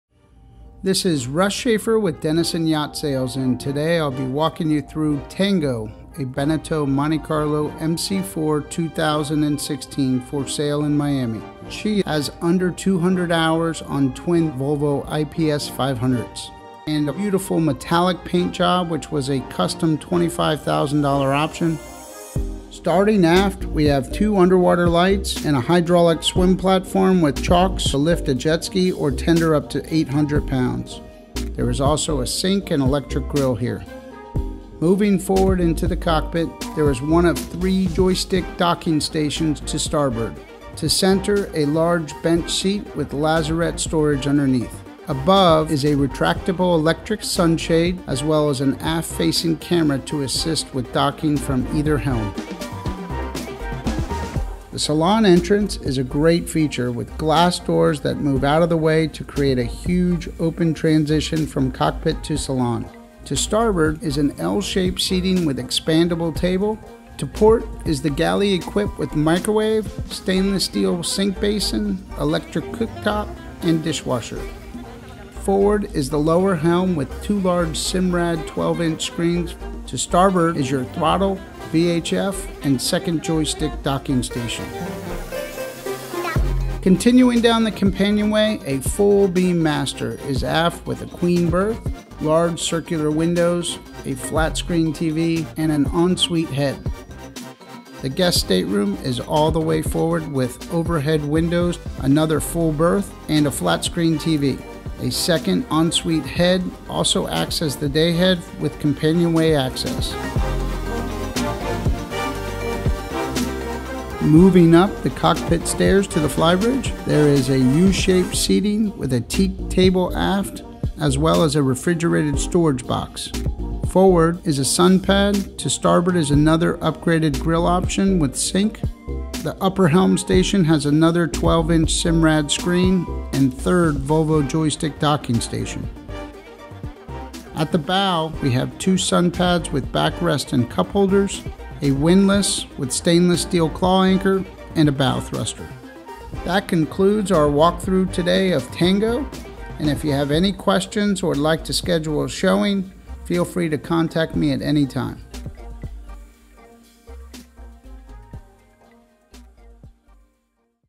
Monte Carlo MC4 Yacht Walkthrough [$598,000]
45' Beneteau 2016 [Tango]

'Tango' is a well-maintained Beneteau Monte Carlo 4 loaded with factory options. Her Volvo IPS500's have under 200 hours. If you're in the market for a 2 stateroom/2 head flybridge cruiser, make sure you get onboard.

Key Features + Options Include:

-Volvo IPS 500 with 3 Joystick Docking Stations
-Onan Generator (190hrs as of 8/18)
-Metallic Paint Job ($20K Option)
-Hydraulic Swim Platform with Grill and Sink Option
-Electric Sunshade
-Full Simrad Electronics: 2x NS12 lower helm; 1x NS12 in Flybridge; Radar, Depth, Autopilot with Remote
-SAT TV
-Bowthruster; Windlass
-Full Beam Master
-Dishwasher; Washer/Dryer Combo

0:00 Introduction
0:26 Swim Platform + Cockpit
0:54 Salon + Galley
1:16 Main Helm
1:29 Master Stateroom
1:38 Guest Staterooms
1:57 Flybridge
2:18 Bow
2:26 Outro